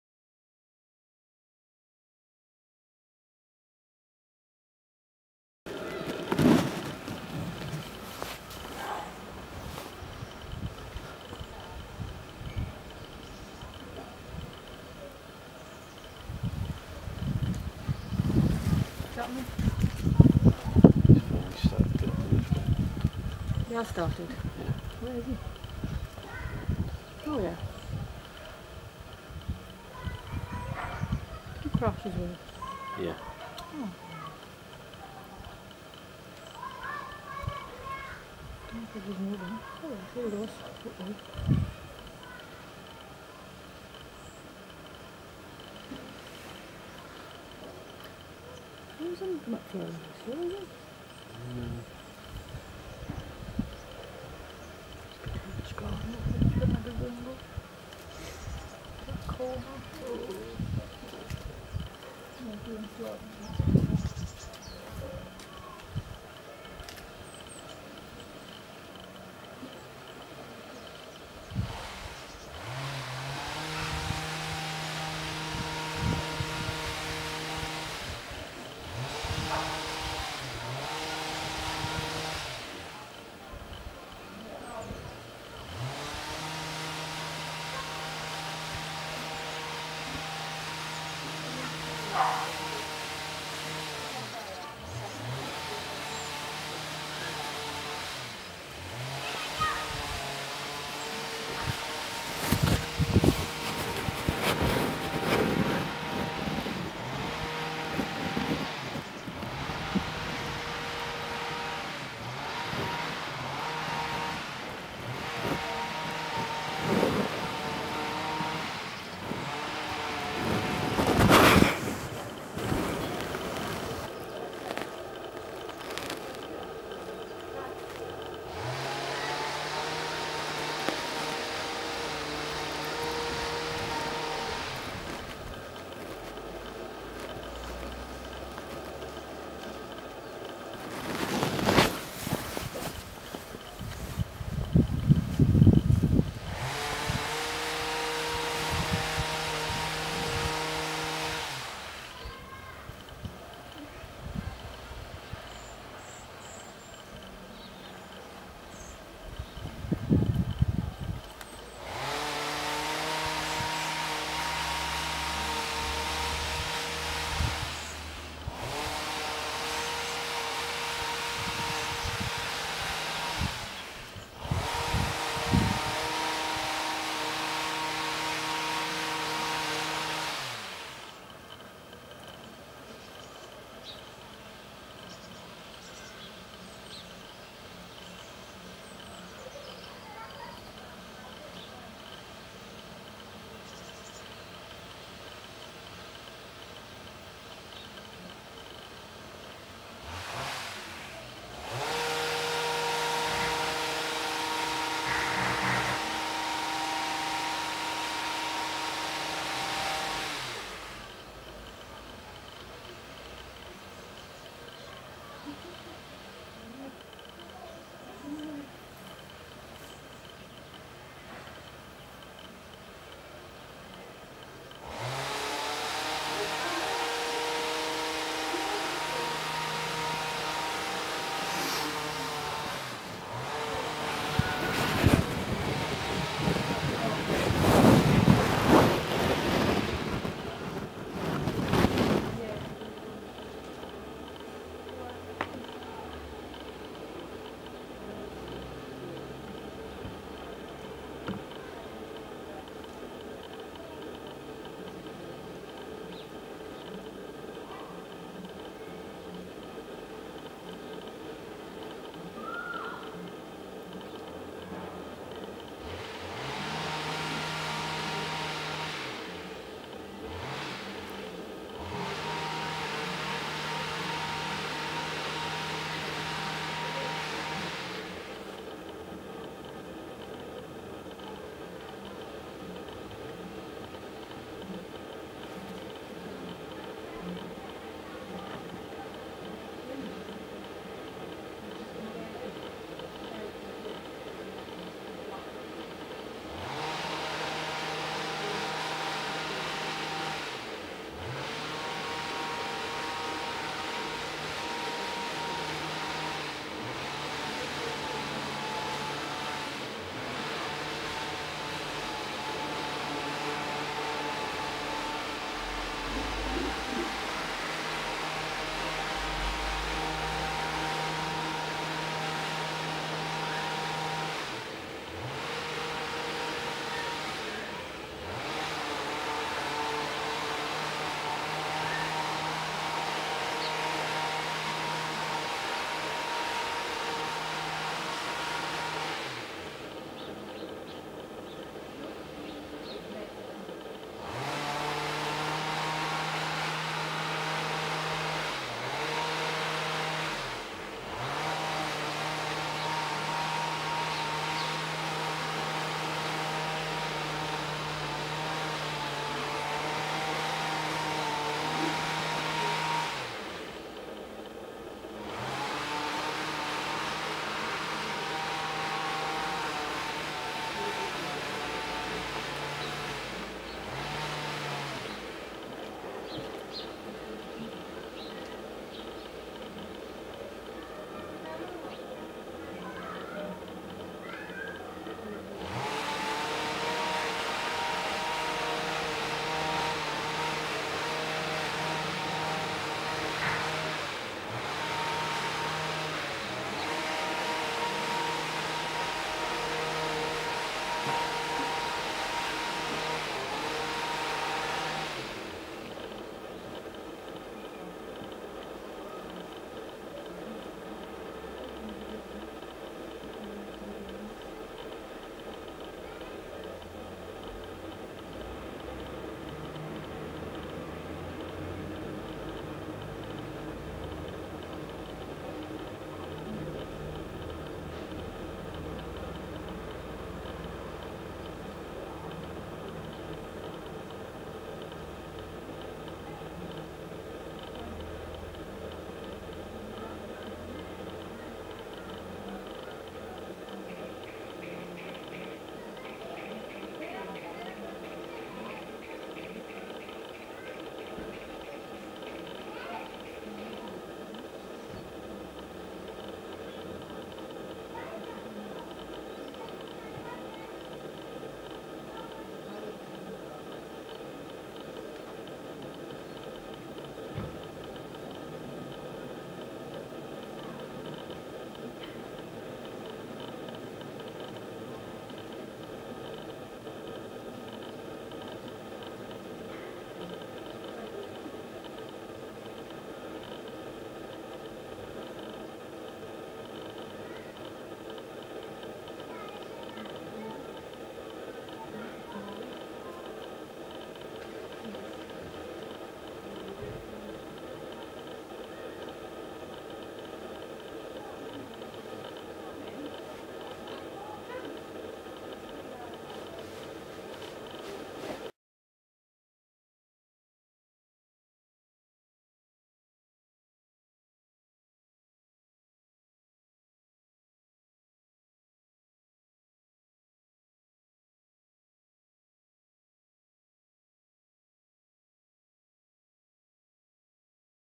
The Spiders Web.wmv
Video of a spider constructing a web